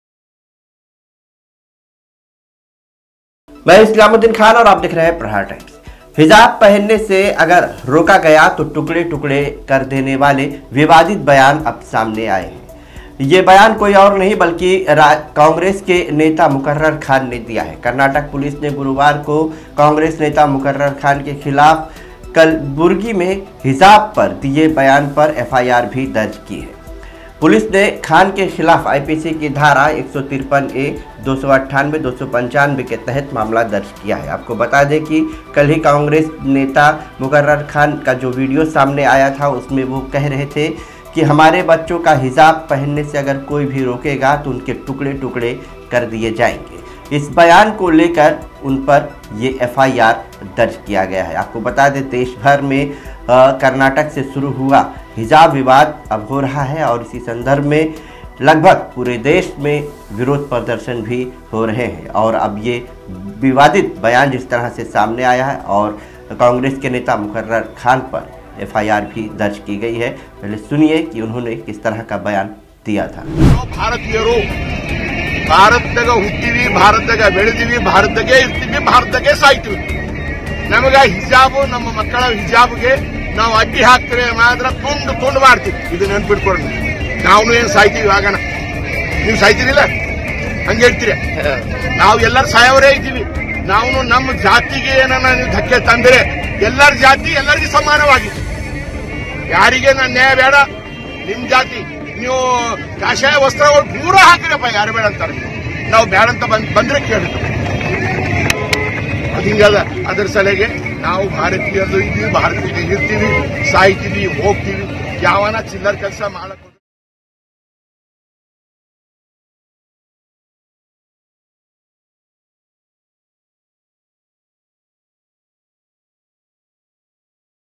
हिज़ाब पहनने से रोका तो टुकड़े टुकड़े कर देंगे, कॉग्रेस नेता का बयान || Islamuddim Khan|| Prahar Times
हिज़ाब पहनने से रोका तो टुकड़े टुकड़े कर देंगे, कॉग्रेस नेता का बयान || Islamuddim Khan

हिज़ाब पहनने से अगर रोका तो टुकड़े टुकड़े कर देने वाले बयान सामने आया है ये बयान कोई और नही बल्कि कॉंग्रेस के नेता मुक़र्रर खान ने दिया है
कर्नाटक पुलिस ने गुरुवार को कांग्रेस नेता मुकर्रम खान के खिलाफ कलबुर्गी में हिजाब पर दिए बयान पर एफआइआर दर्ज की है। पुलिस ने खान के खिलाफ आईपीसी की धारा 153 (ए), 298, 295 के तहत प्राथमिकी दर्ज की। बता दें कि कल ही कांग्रेस नेता का जो वीडियो सामने आया उसमें वो कह रहे थे कि 'जो भी हमारे बच्चों को हिजाब पहनने से रोक रहे हैं, उनके टुकड़े-टुकड़े कर दिए जाएंगे।' इस बयान को लेकर ही उन पर केस दर्ज किया गया है